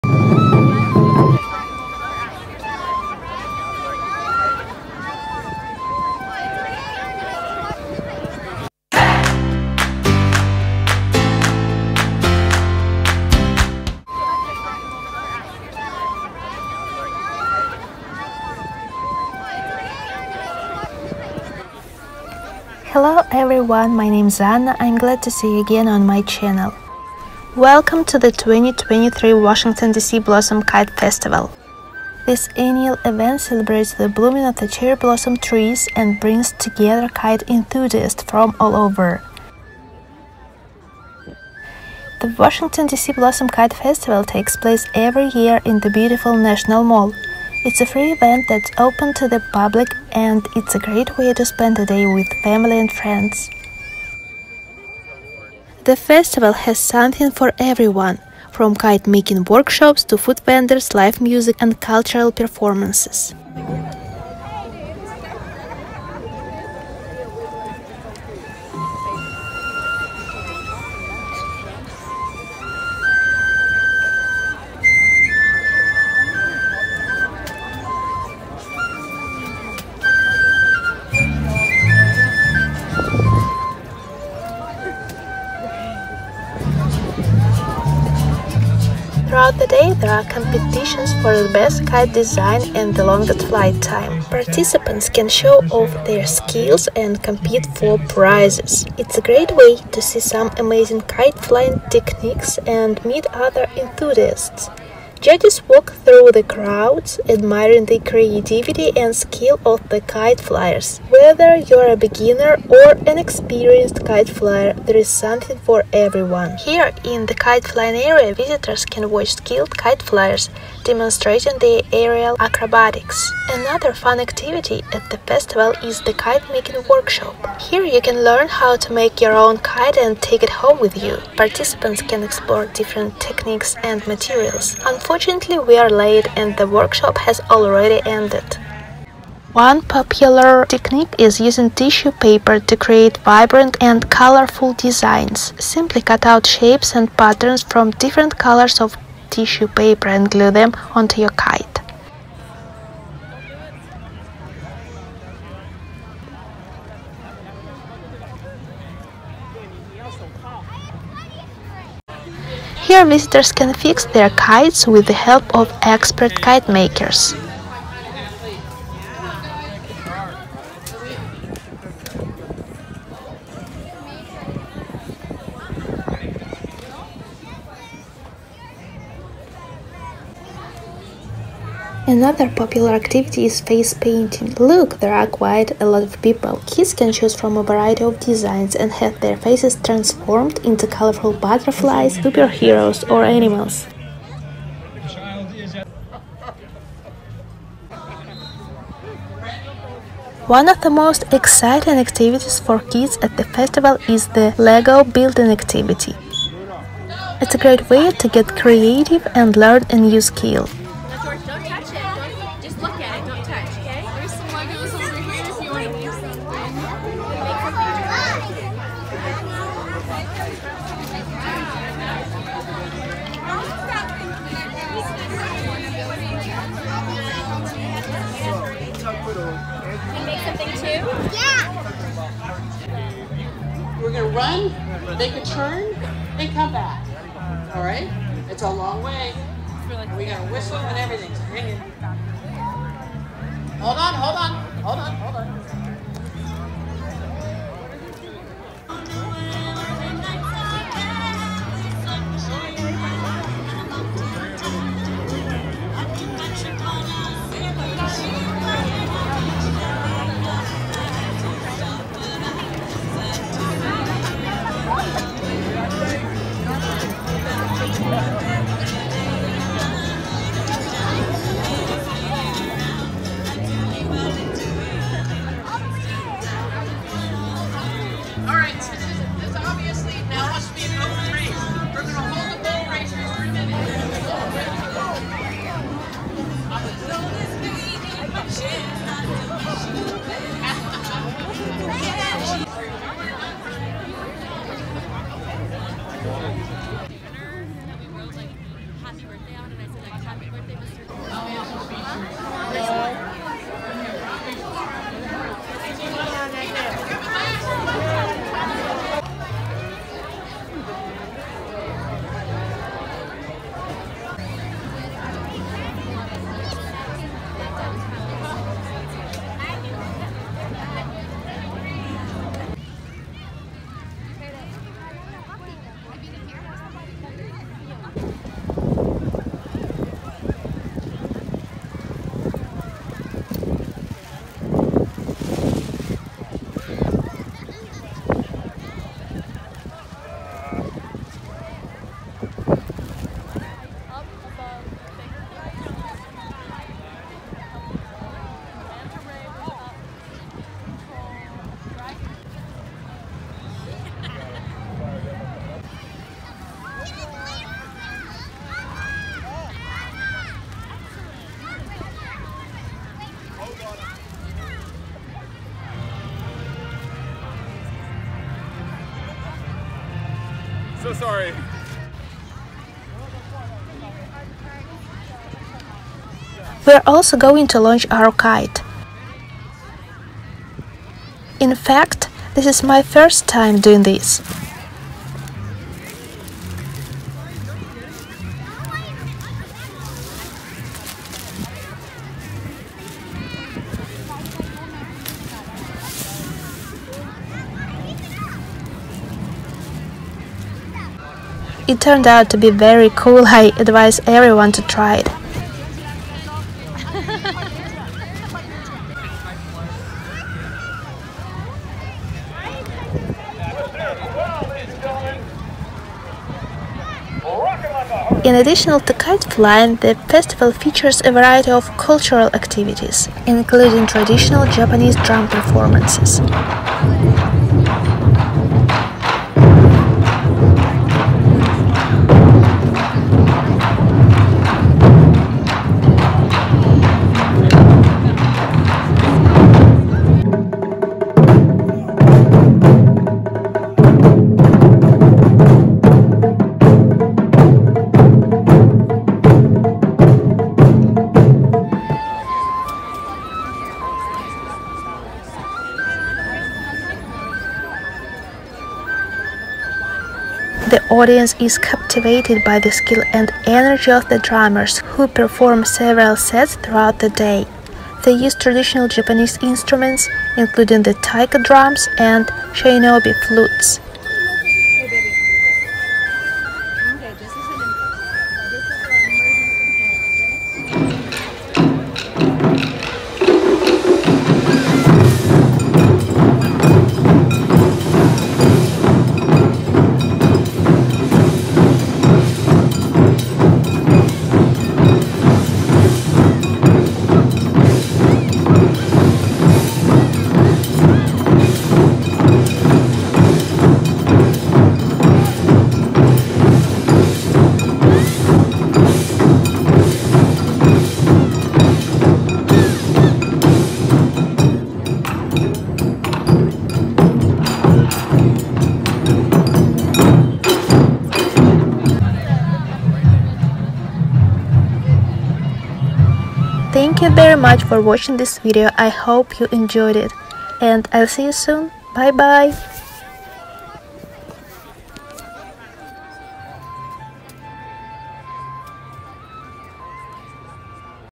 Get Crafty and Fly High at the 2023 Washington D C Blossom Kite Festival!
Welcome to the 2023 Washington D.C. Blossom Kite Festival! In this video, I take you on a tour of this incredible annual event that celebrates the blooming of the cherry blossom trees and brings together kite enthusiasts from all over.
Watch as we explore the festival grounds, from the kite-making workshops to the food vendors, live music, and cultural performances. You'll also see some amazing kite-flying techniques and meet other enthusiasts as we check out the competitions for the best kite design and the longest flight time.
In addition to kite-flying, the festival features a variety of cultural activities, including traditional Japanese drum performances. You'll be captivated by the skill and energy of the drummers as they perform several sets throughout the day.
But that's not all - there's something for everyone at the Washington D.C. Blossom Kite Festival! Kids can choose from a variety of designs and have their faces transformed into colorful butterflies, superheroes, or animals through face-painting. Plus, the LEGO building activity is a great way for kids to get creative and learn a new skill.
And of course, we can't forget about launching our own kite! Watch as I try kite-flying for the first time and see how it turns out.
Overall, the Washington D.C. Blossom Kite Festival is an incredible event that's open to the public and free of charge. It's a great way to spend a day with family and friends and celebrate the beauty of spring. So come join me and let's get crafty and fly high together!